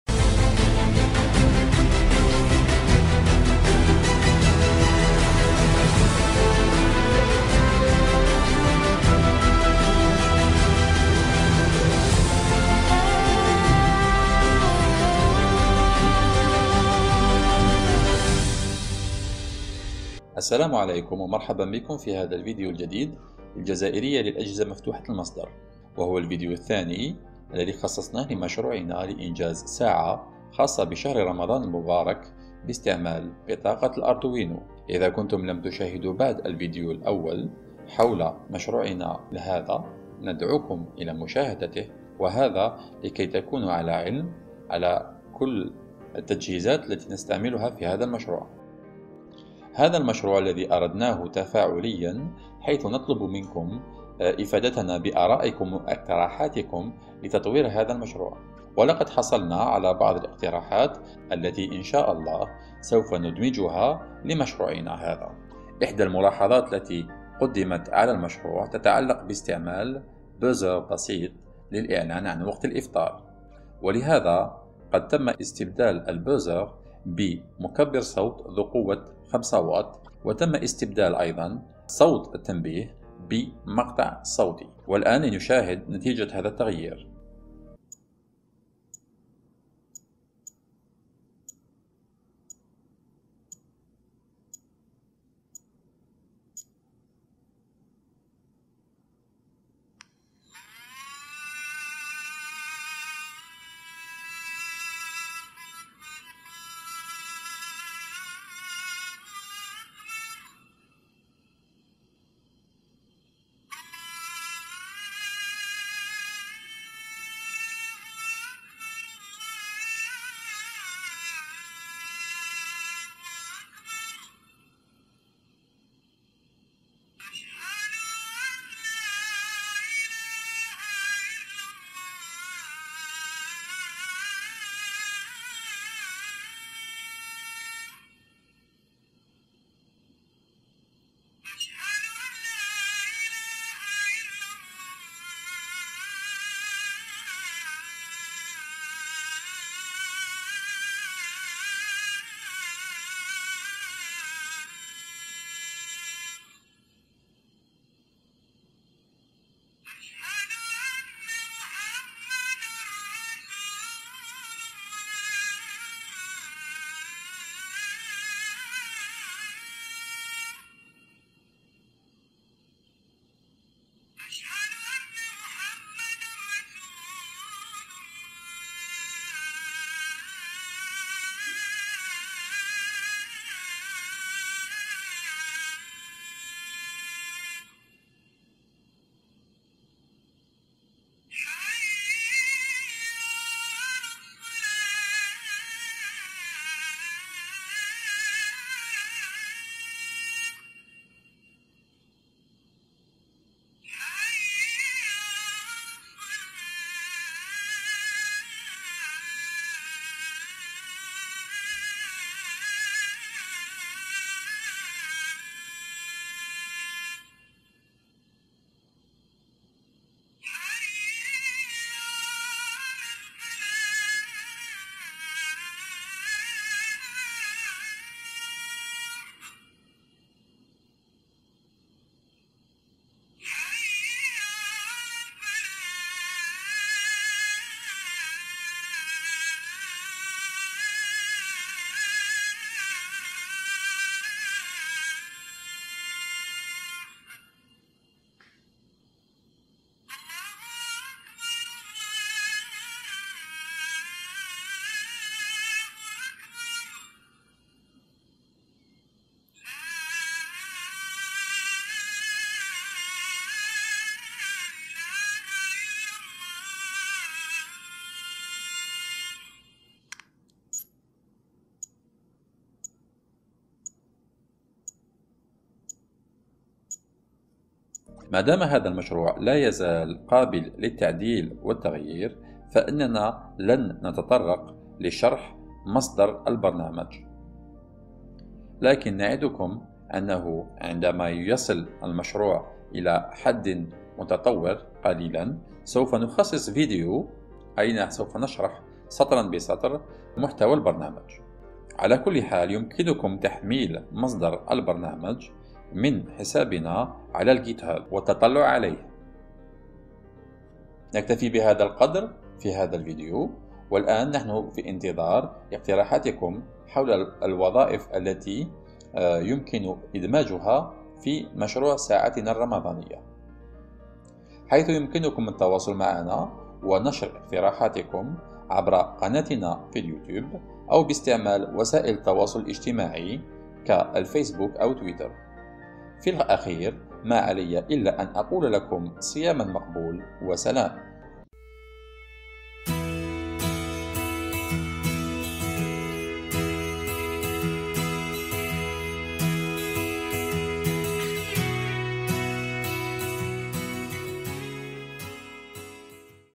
مشروع ساعة رمضانية بإستعمال الاردوينو‬ (الجزء 2)
ها نحن هنا مرة أخرى بمفهوم جديد لمشروع أردوينو ، وهو مشروع تتاح لك فرصة المشاركة فيه من خلال مشاركة آرائك واقتراحاتك حول تطور المشروع ساعة رمضانية   بإستعمال الاردوينو‬ .